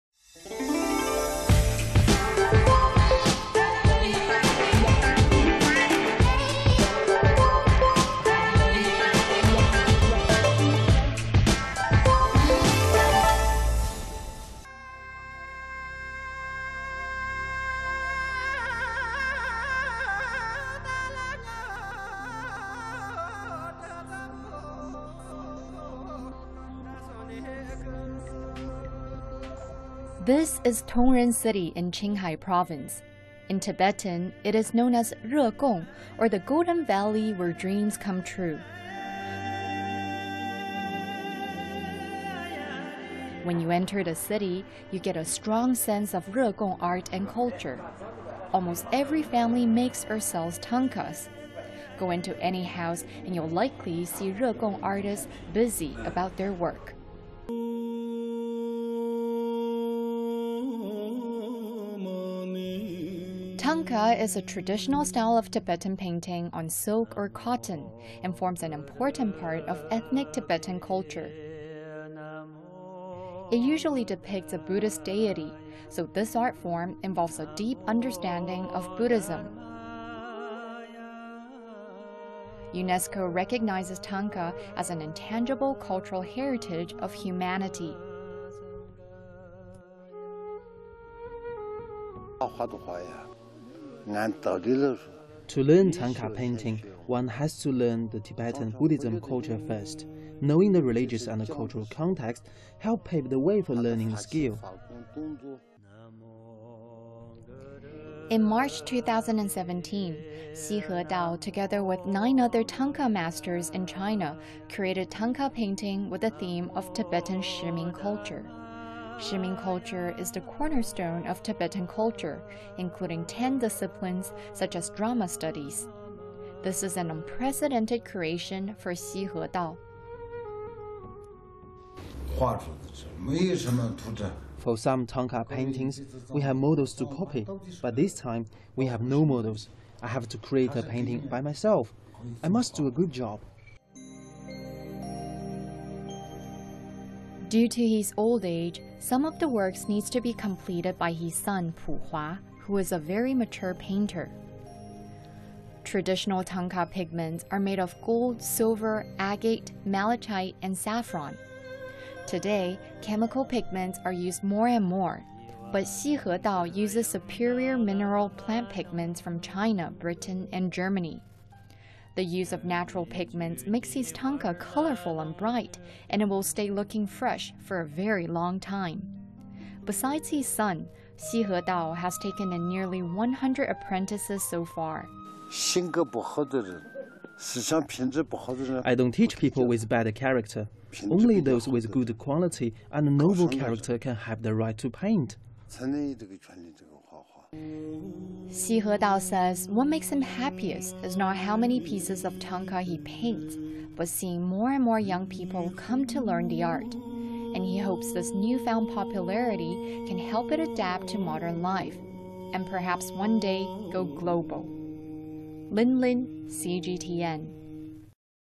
Tibetan art-form slowly gaining more followers
Thangka, or Tibetan Buddhist story painting, is a striking element of Tibetan culture. It has been developing for thousands of years, but like many time-honored traditions, it's looking for ways to compete in the 21st century. One master painter is hoping the younger generation can incorporate some modern elements to keep it alive.